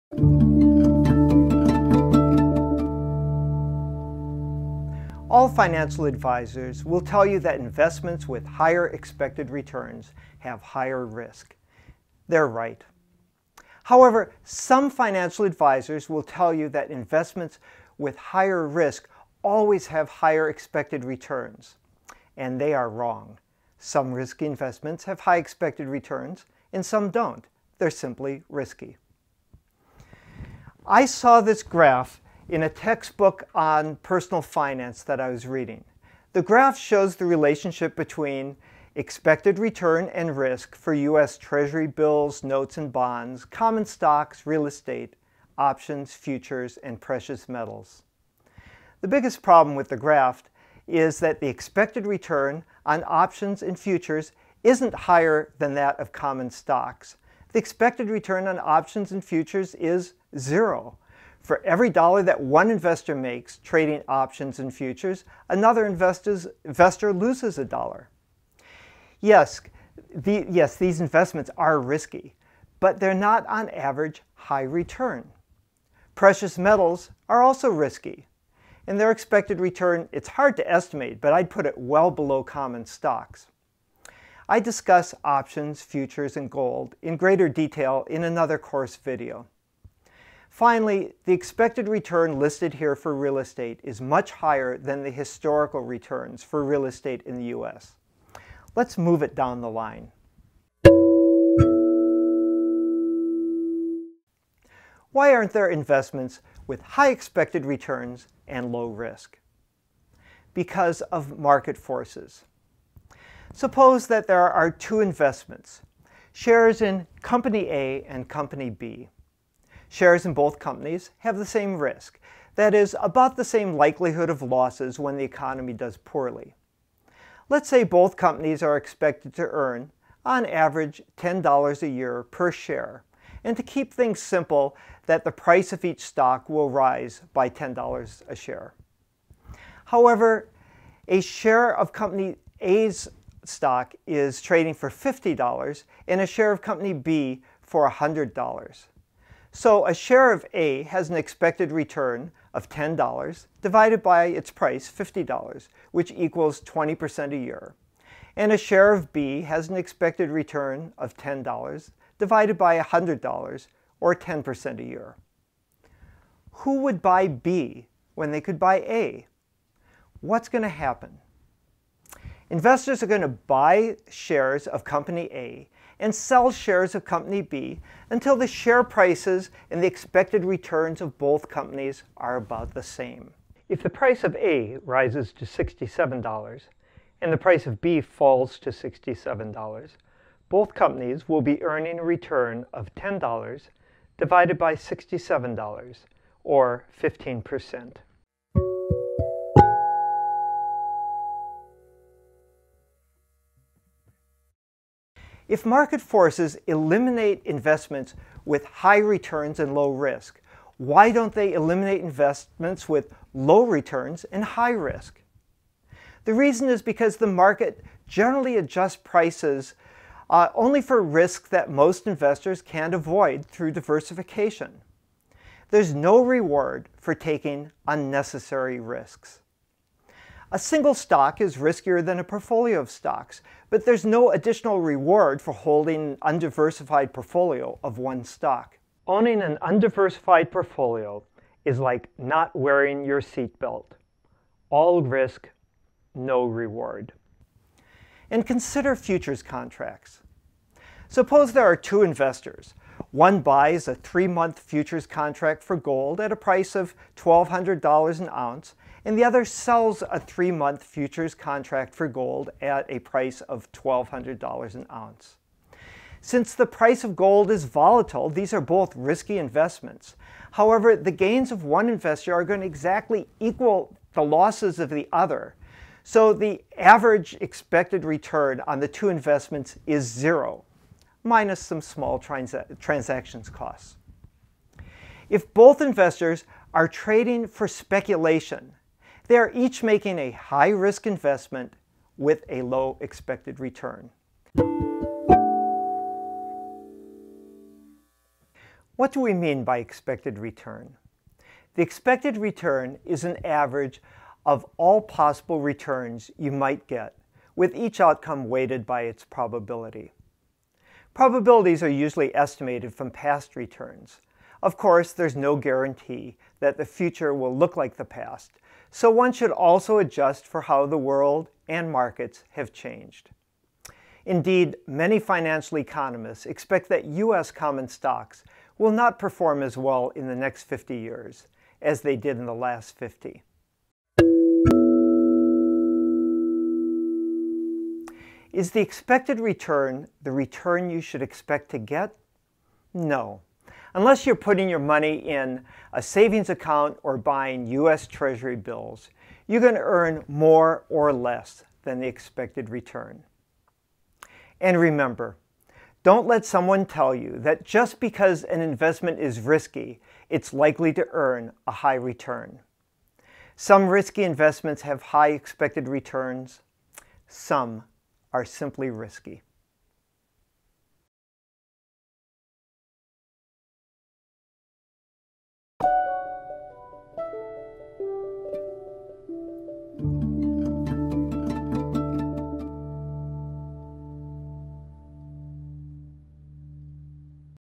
Risk and Return
the relationship of risk and return in financial markets